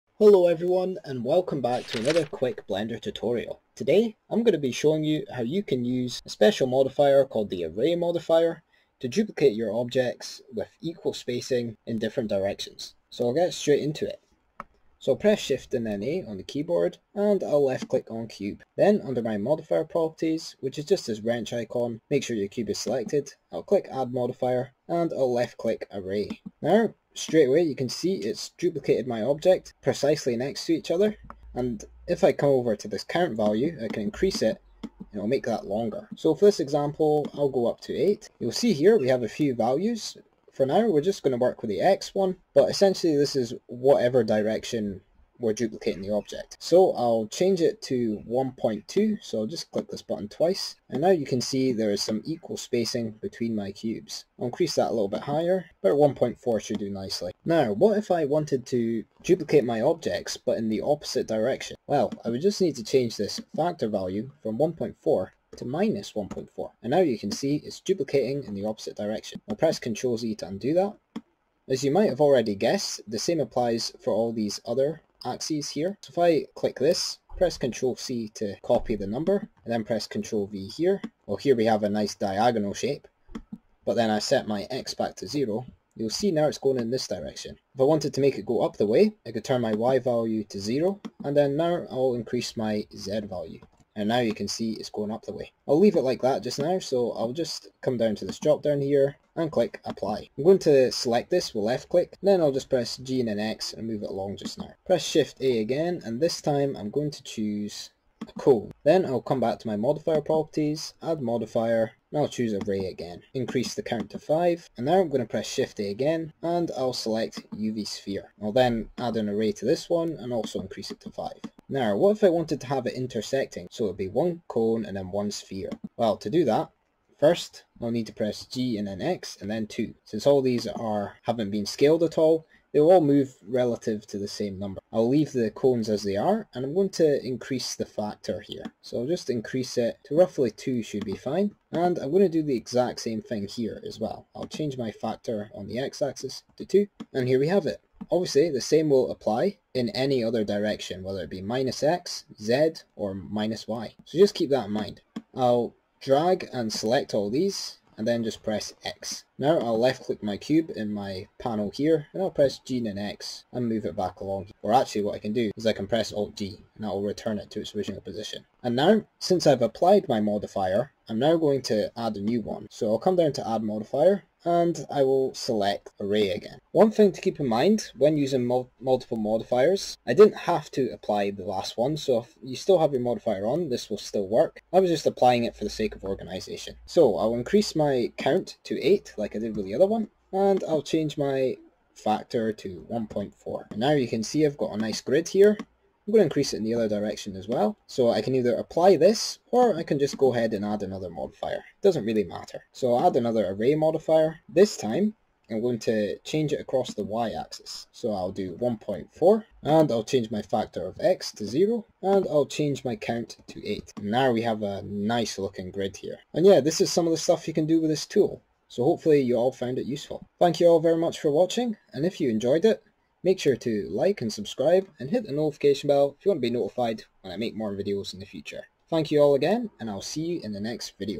DUPLICATE OBJECTS WITH EQUAL SPACING - BLENDER QUICK TIPS
How to use the array modifier in blender!

Sound Effects/Music: Filmora Studio
Editing software: Filmora Studio